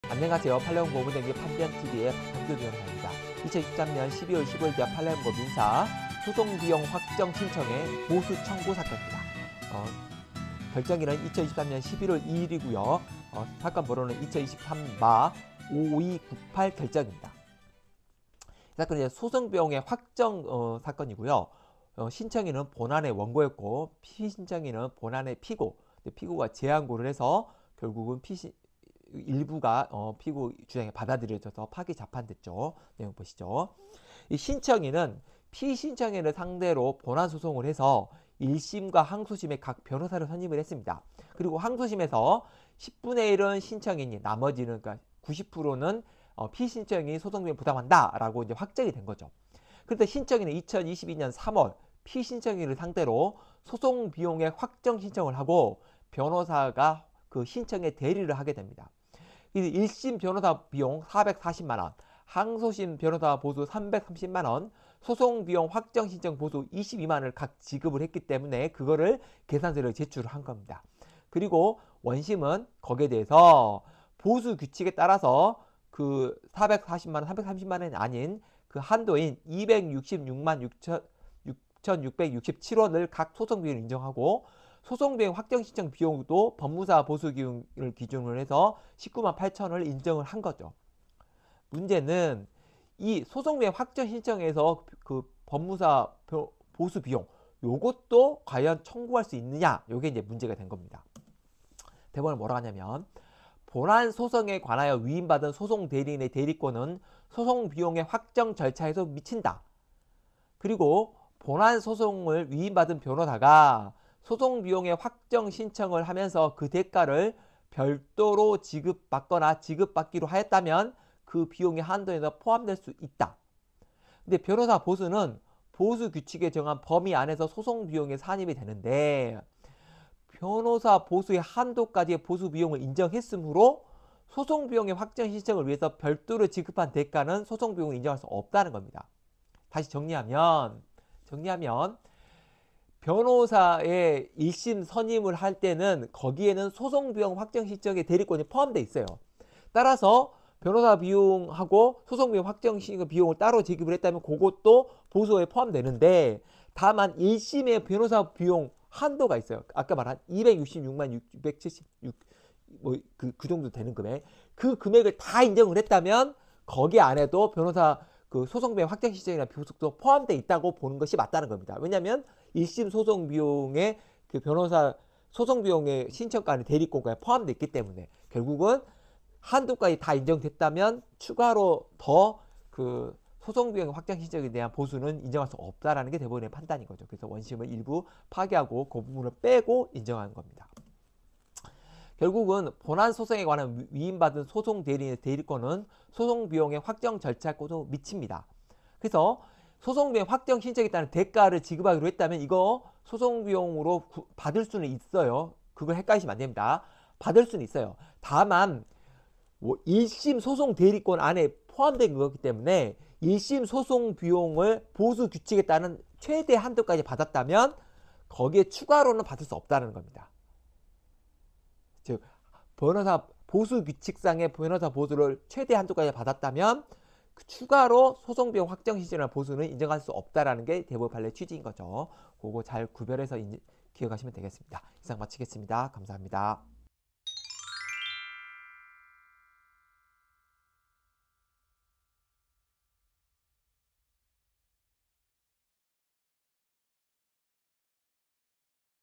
소송비용확정신청의 보수 사건 [23.12.15.자 판례공보(민사)]
변호사와 법조 실무자를 위한 판례공보 5분 요약입니다.

소송비용확정신청의 보수 청구 사건
별 1개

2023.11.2. 자 2023마5298 결정
소송비용액확정

신청인  본안의 원고
피신청인 본안의 피고(재항고인)

파기자판

신청인은 피신청인을 상대로 본안소송에서 1심, 항소심에서 각 변호사를 선임, 항소심판결에서 1/10은 신청인이, 나머지는 피신청인 부담으로 확정됨
신청인은 22.3 피신청인을 상대로 소송비용액 확정신청을 하고, 변호사가 신청인 대리를 함
1심 변호사 보수 440만원, 항소심 변호사 보수 330만원, 소송비용확정신청 보수 22만원를 각 지급, 그 소송비용계산서를 제출
원심은 1심, 2심을 보수규칙에 따라 각 2,666,667원을 소송비용으로 인정하고, 소송비용액 확정신청 비용도 198,000원(법무사 보수기준) 인정함

소송비용 확정신청에서 1심과 2심 각 변호사 보수 외에 소송비용액 확정신청의 변호사비용도 청구할 수 있는가

본안소송에 관하여 위임받은 소송대리인의 대리권은 소송비용액 확정절차에도 미침
본안소송을 위임받은 변호사가 소송비용액 확정신을 하면서 그 대가를 별도로 지급받거나 지급받기로 하였다면 그 비용의 한도에서 포함될 수 있음
변호사보수는 보수규칙에 정한 범위 안에서 소송비용에 신입됨
변호사보수의 한도까지 보수비용을 인정했으므로, 소송비용액 확정신청을 위하여 별도로 지급한 대가는 소송비용으로 인정될 수 없음

실무활용
소송비용 확정 신청에 따른 별도의 대가 지급을 한 경우, 그 비용도 소송비용으로 청구할 수 있음
다만 소송비용 확정을 위한 변호사 보수비용의 한도 내에서 인정됨
보수규칙의 변호사보수로 한도까지 인정했으므로, 소송비용액 확정신청을 위한 보수는 인정하지 않은 것